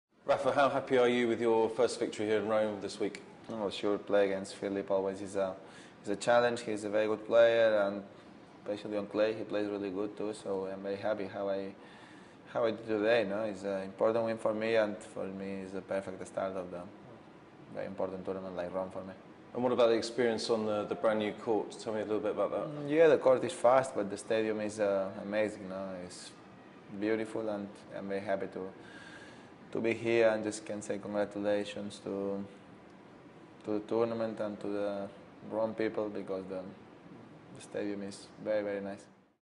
Rafael Nadal Wednesday Interview Rome 2010
Rafael Nadal talks after his opening-round win Wednesday in Rome.